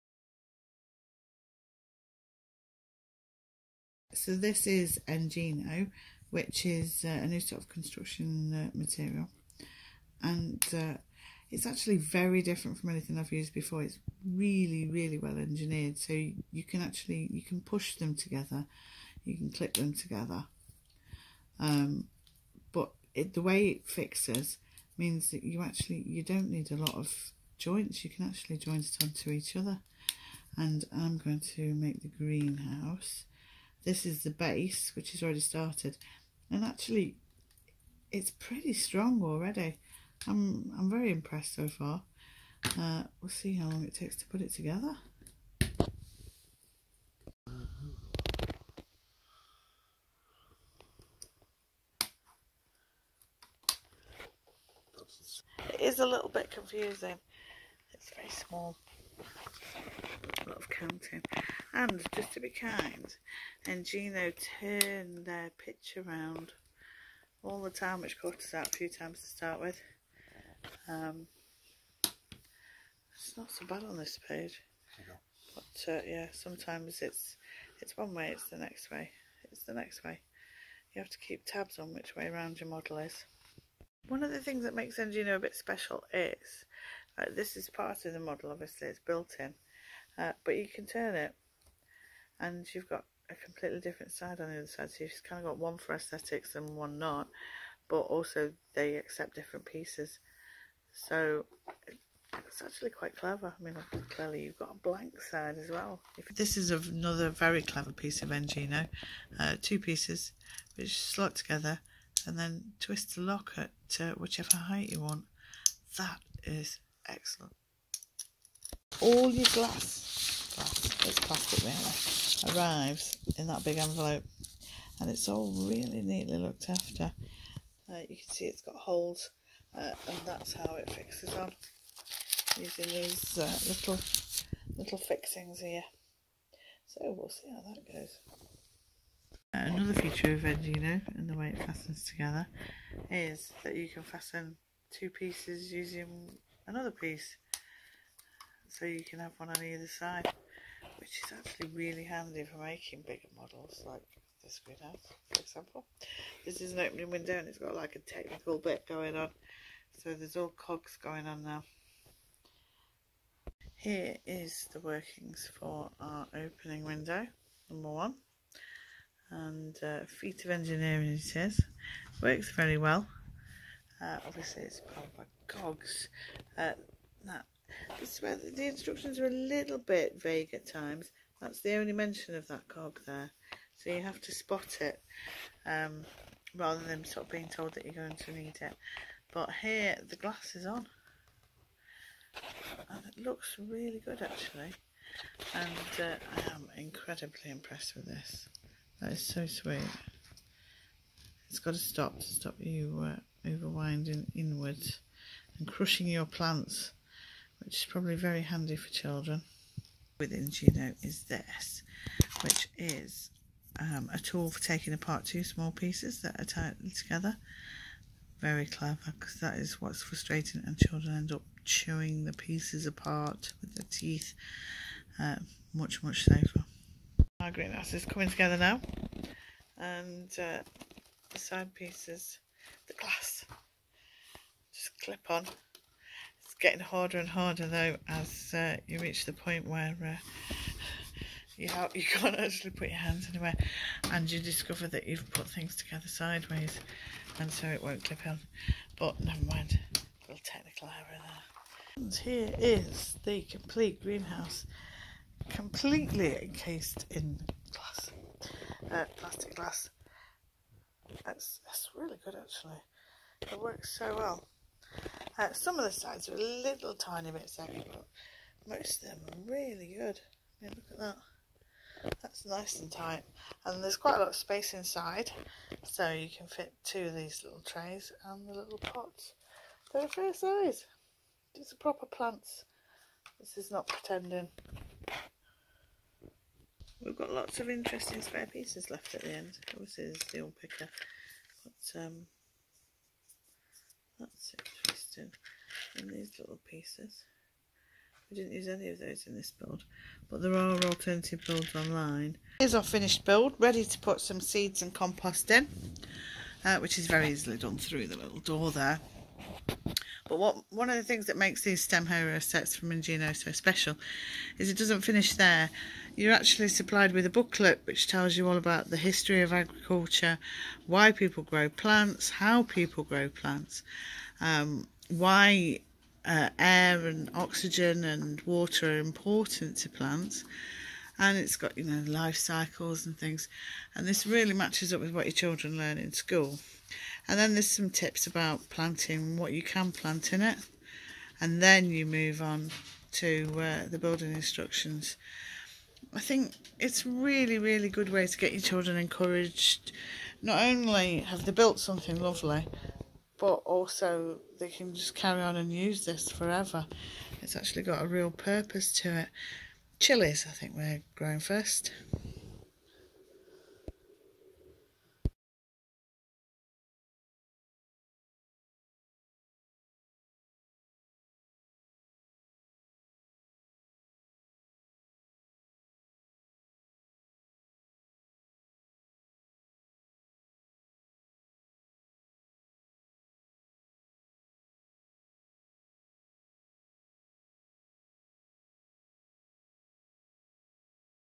Engino STEM Heroes Greenhouse Construction Toy Review Age 8+ Sent for review by Engino
Engino sent us one of the new STEM Heroes sets for review - The Greenhouse (age 8-14+). An afternoon's build and years of growing! The Greenhouse is a genuine working greenhouse with an opening door and 2 controlled opening windows. Supplied with 12 pots and 2 seedling trays.
All Engino construction sets use a unique design which is very quick, minimises fixings and sets can all be combined.  Full review as ever on the blog.
(I wasn't paid for this review/video.)